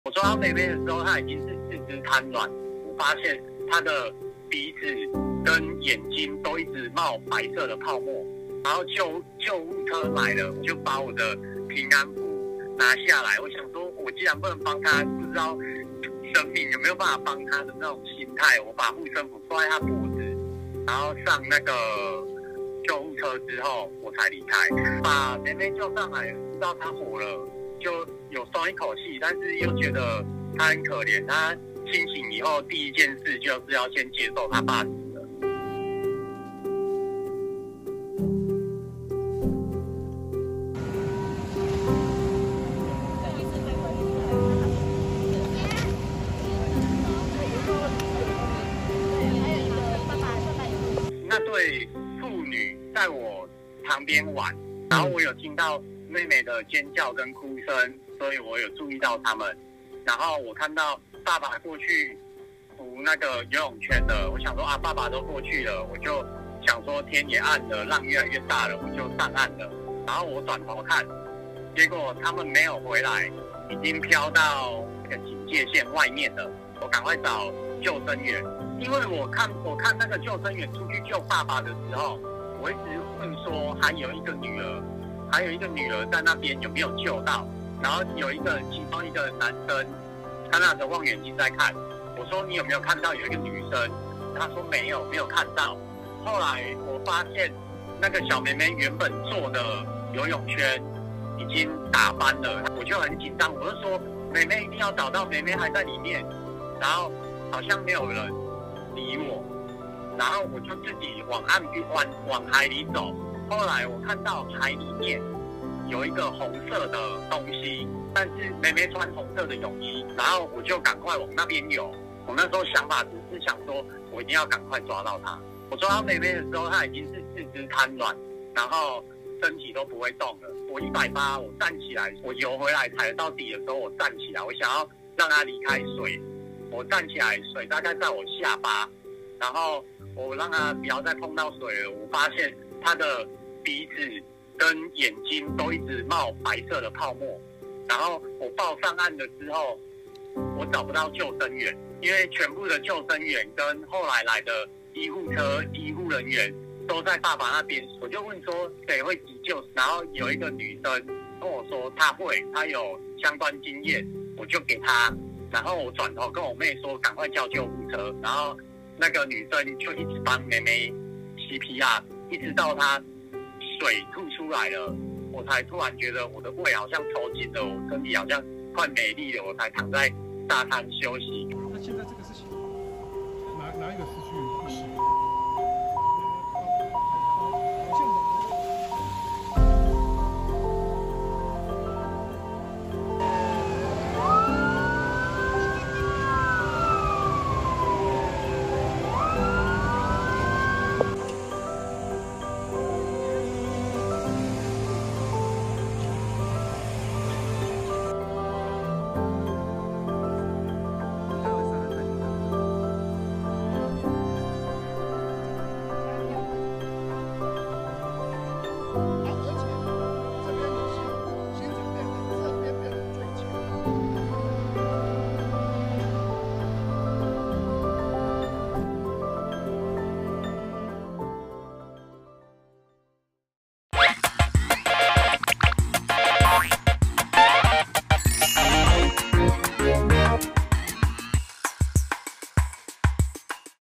大武崙溺水女童七孔冒泡 勇男游80m救人送平安符
獨／大武崙溺水女童七孔冒泡！勇男游80m救人　心疼1事送她平安符